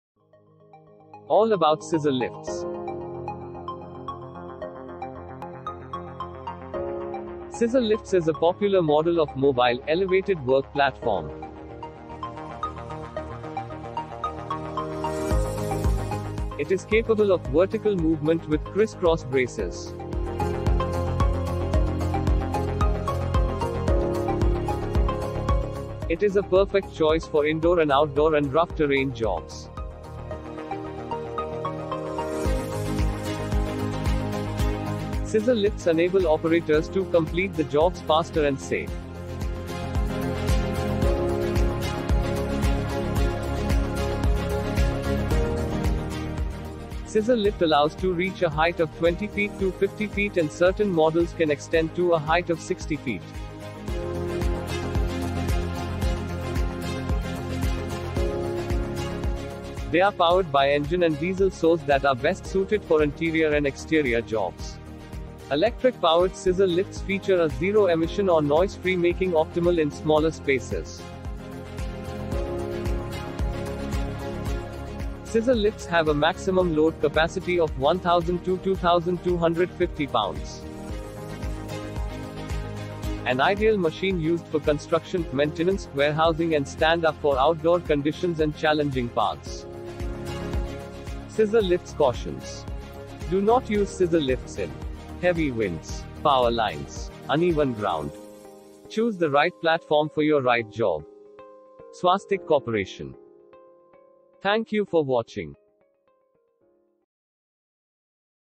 Things you should know about scissorlift | Aerial Work Platform
An ideal mobile elevated work platform for use in indoor and outdoor jobs, to work safely above ground.

In this video, you will know all about ScissorLift, is a piece of the machine from SwastikCorporation offers equipment from our fleet.

A Tour to ScissorLifts

0:02 - Introduction
0:08 - About Scissor Lift
0:17 - Moves in Vertical Direction
0:26 - Indoor & Outdoor Jobs
0:36 - Work Faster & Safe
0:46 - Max Height To Reach
1:00 - Powered by Engine & Diesel Source
1:06 - Best use of Electric Power Source
1:15 - Max Load Capacity
1:23 - Industry Applications
1:30 - Scissor Lift cautions

Check Out to get the best price for your job work.